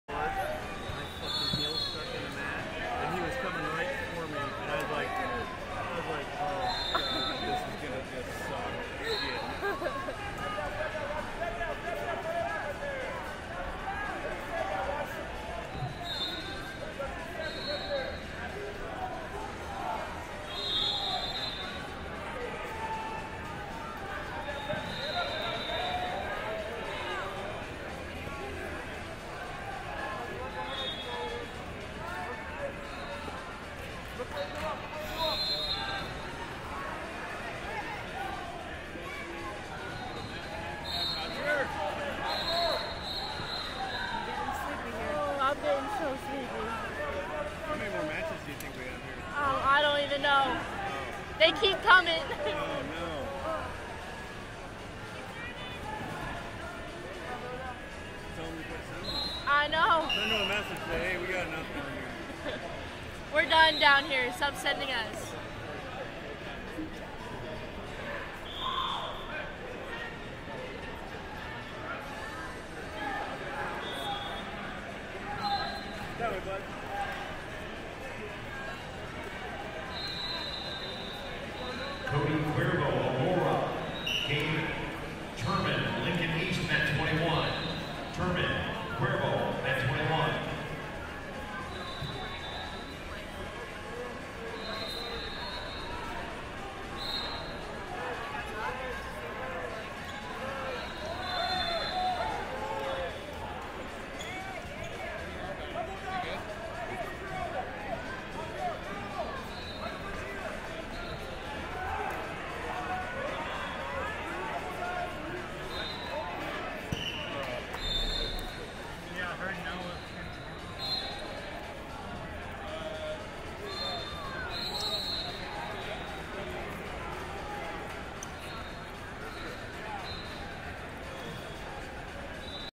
High School (9th - 10th Grade) 220 Robert Beese Amery Warriors Vs Roger Lyall Moline WC
2019 Preseason Nationals - High School (9th - 10th Grade) 220 Robert Beese Amery Warriors Vs Roger Lyall Moline WC